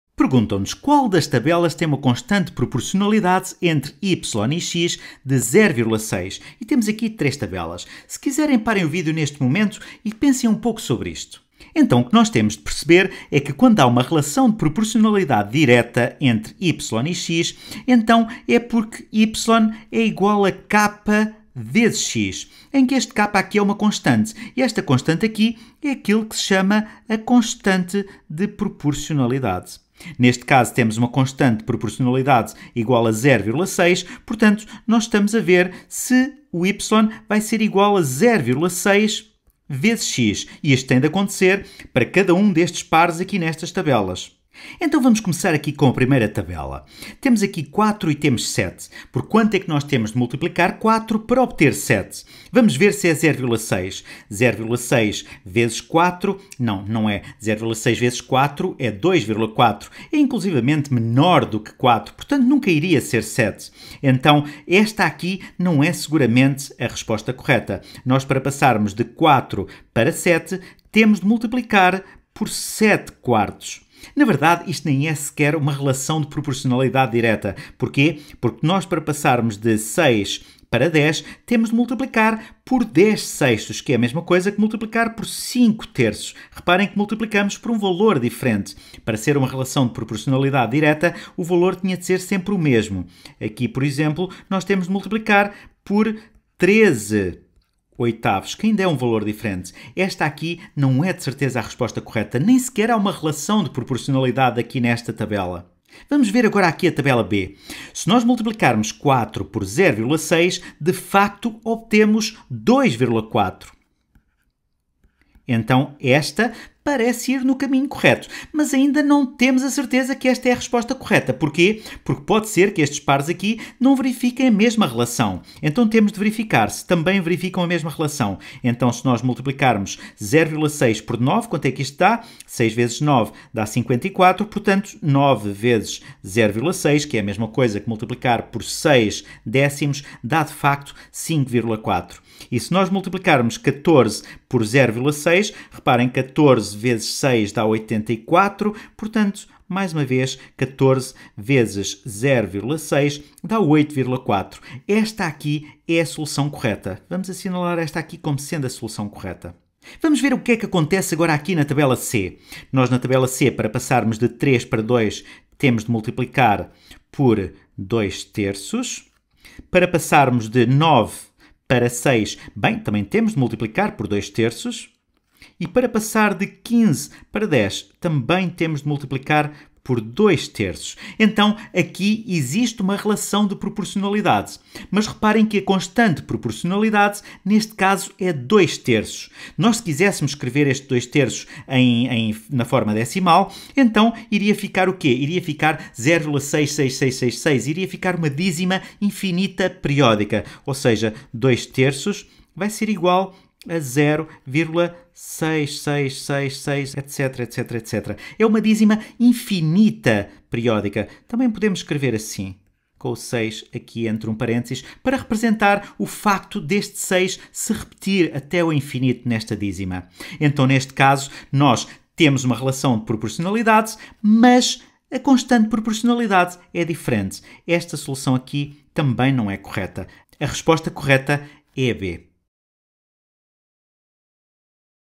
Constante de proporcionalidade a partir de tabelas - Khan Academy em português (7.º ano)
Vídeo original: Constant of proportionality from tables